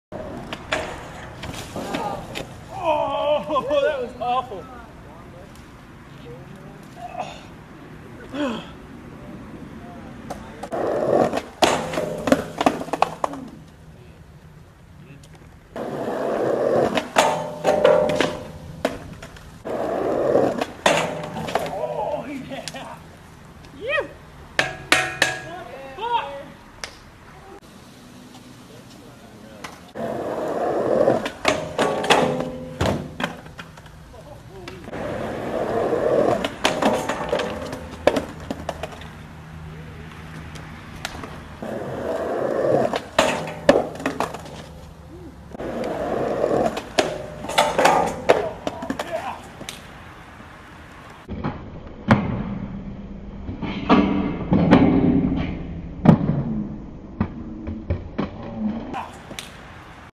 Shreddit police car railslide attempt fails
A guy makes several attempts to rail slide in front of a police car but each time smacks onto the rail or the concrete.